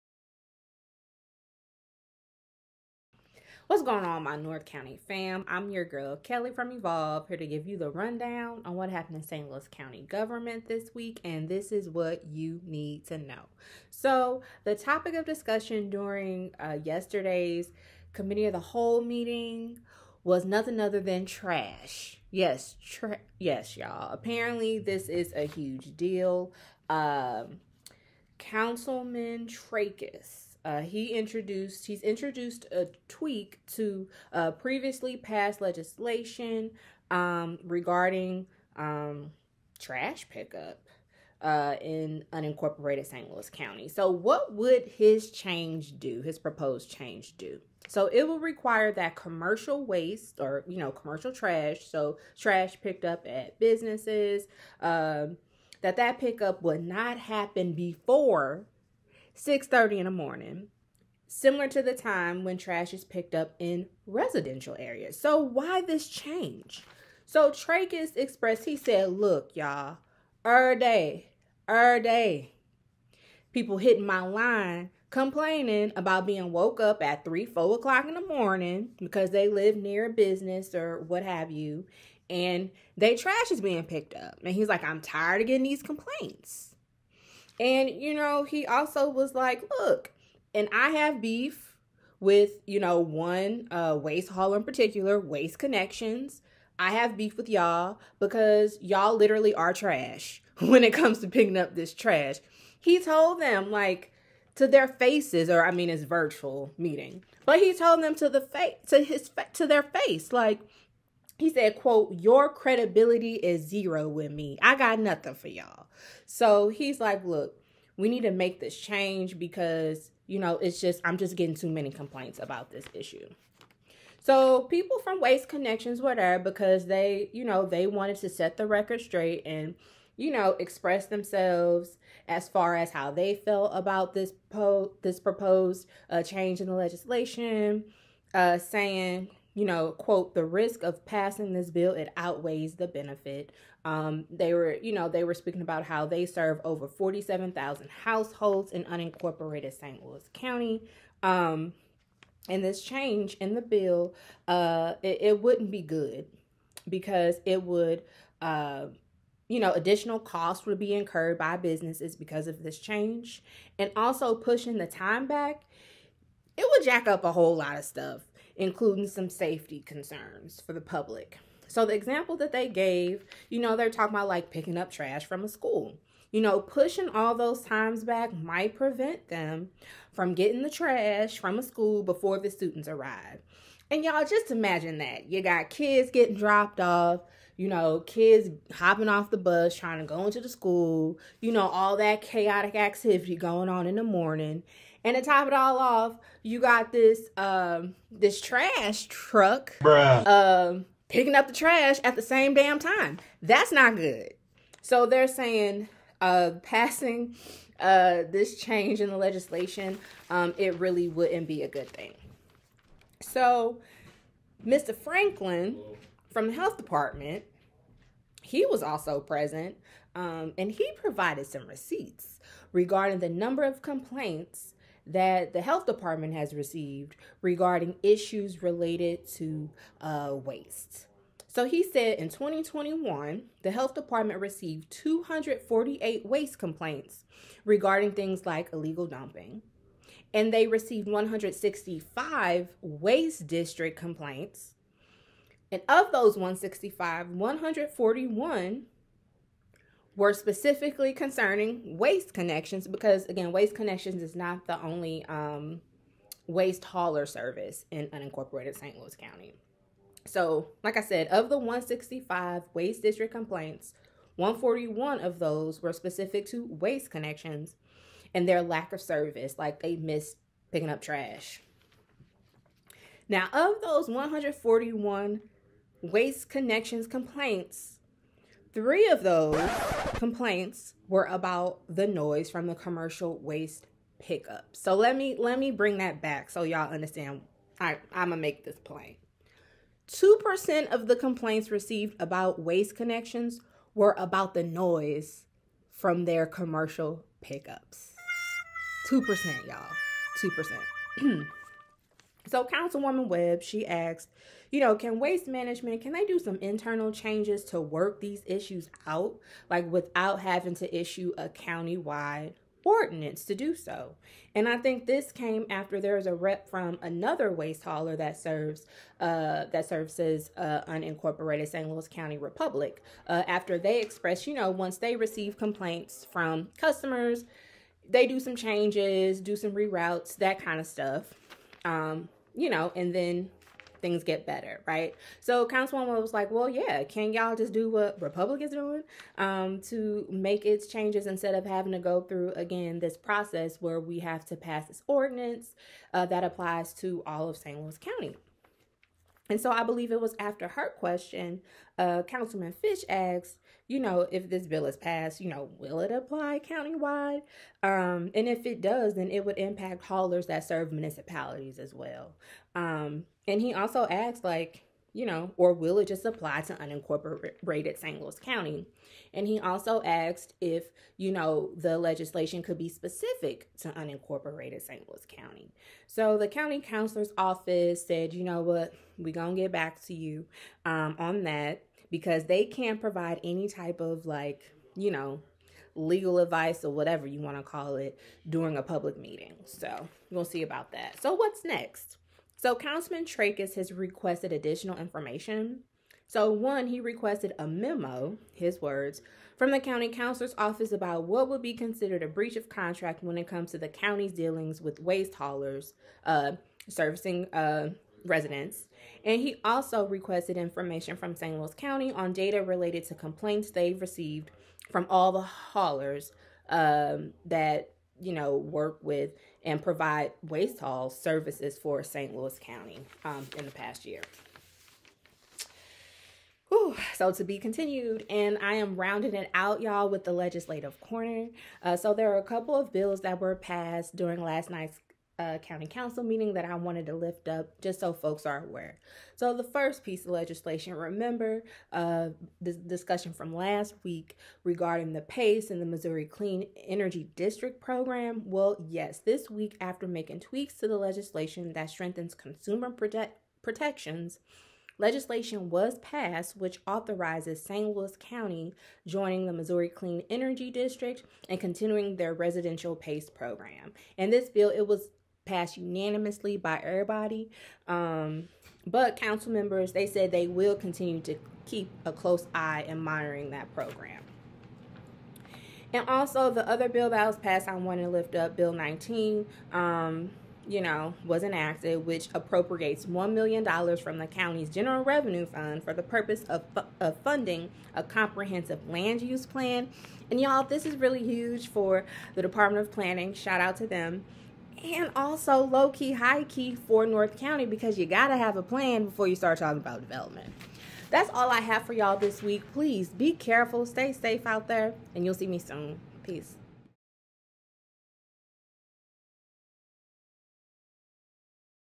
ICYMI: STL County | 2% | Wk of 1.31.22
In case you missed it, Kelly provides a rundown of what happened in St. Louis County government this week.

The main topic of discussion...? TRASH!

Check out the video, and let us know how y'all are feeling about this topic.